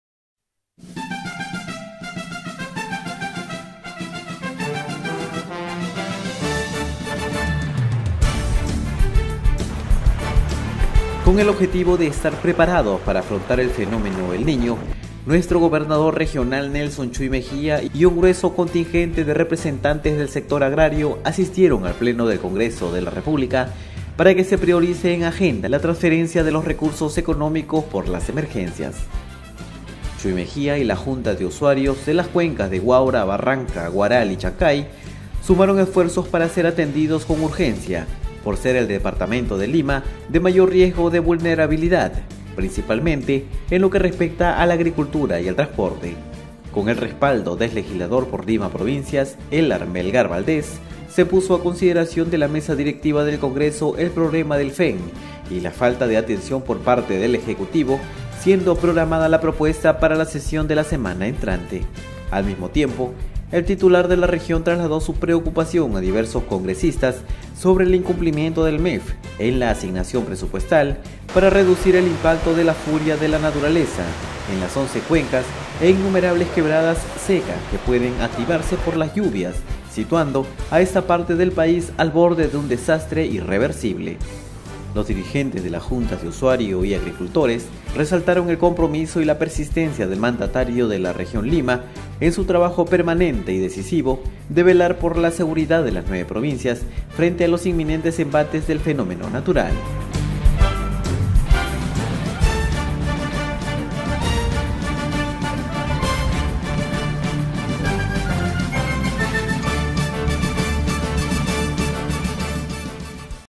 SUMAN ESFUERZOS PARA PRIORIZAR EN AGENDA DEL CONGRESO TRANSFERENCIA DE RECURSOS
Gobernador Nelson Chui y Junta de Usuarios del Agua de la región Lima asistieron a sesión ordinaria

SUMAN ESFUERZOS PARA PRIORIZAR EN AGENDA DEL CONGRESO TRANSFERENCIA DE RECURSOS POR FENÓMENO EL NIÑO

Con el objetivo de estar preparados para afrontar el fenómeno “El Niño”, nuestro gobernador regional Nelson Chui Mejía y un grueso contingente de representantes del sector agrario asistieron al Pleno del Congreso de la República para que se priorice en agenda la transferencia de los recursos económicos por las emergencias.

Chui Mejía y las Juntas de Usuarios de las cuencas de Huaura y Barranca, Huaral y Chancay sumaron esfuerzos para ser atendidos con urgencia, por ser el departamento de Lima el de mayor riesgo de vulnerabilidad, principalmente en lo que respecta a la agricultura y el transporte.

Con el respaldo del legislador por Lima Provincias, Elard Melgar Valdez, se puso a consideración de la mesa directiva del Congreso el problema del FEN y la falta de atención por parte del Ejecutivo, siendo programada la propuesta para la sesión de la semana entrante.

Al mismo tiempo, el titular de la región trasladó su preocupación a diversos congresistas sobre el incumplimiento del MEF en la asignación presupuestal para reducir el impacto de la furia de la naturaleza en las once cuencas e innumerables quebradas secas que pueden activarse por las lluvias, situando a esta parte del país al borde de un desastre irreversible.

Los dirigentes de las juntas de usuarios y agricultores resaltaron el compromiso y la persistencia del mandatario de la región Lima, en su trabajo permanente y decisivo de velar por la seguridad de las nueve provincias frente a los inminentes embates del fenómeno natural.
Huacho, 01 de octubre de 2015